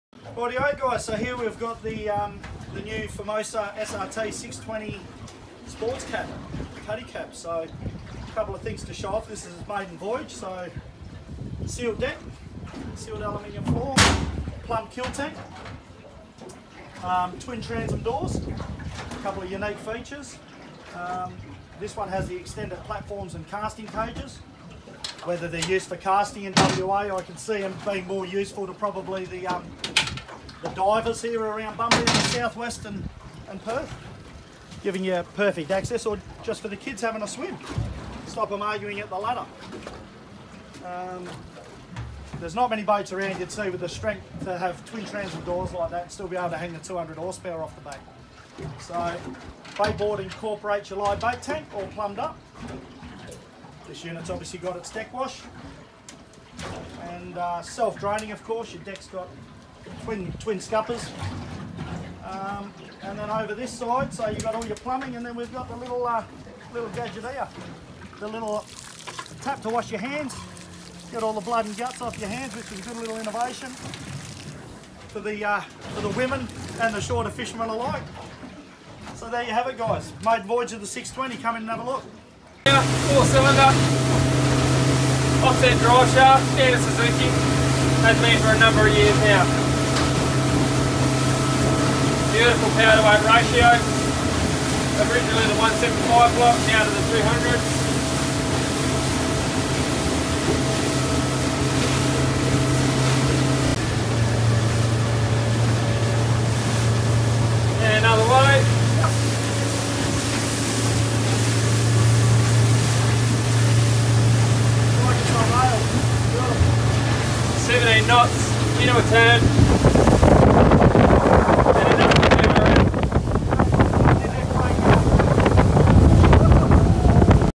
Formosa SRT 620 Cuddy Cab - Millards Walkthrough
Rod Van Oosten from Millards in Bunbury walks through some of the features of the FORMOSA SRT PRO 620 Cuddy Cab - now known as the 635 Cuddy Cab with a new model release coming 2021.

Features include:

- PRO DECK upgrade to a self-draining checker plate floor
- Plumbed Kill Tank
- Dual Access Active Transom WITH Casting Cages
- Max 200HP Suzuki
- Plumbed Bait Tank with Bait Board
- Plumbed Deck Wash AND Hand Wash

The New Formosa SRT Plate Hull combines the very best of Formosa designs packaging them into one skilfully engineered plate boat and branded as Formosa SRT. The new plate hull features the ultimate level of performance, handling and stability, which includes a strong Lock-cell hull and deck structure with a Quad4™ Water Ballast option available across the range.